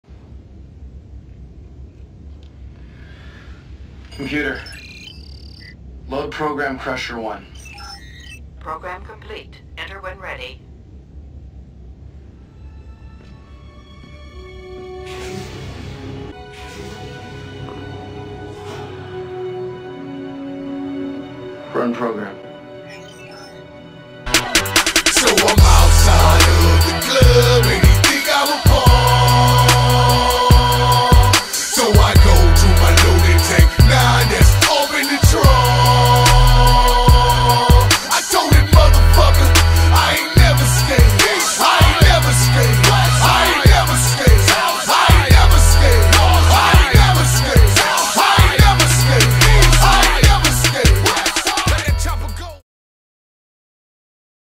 Frog Daddy The Next Generation
Crusher must make a difficult decision and face his destiny.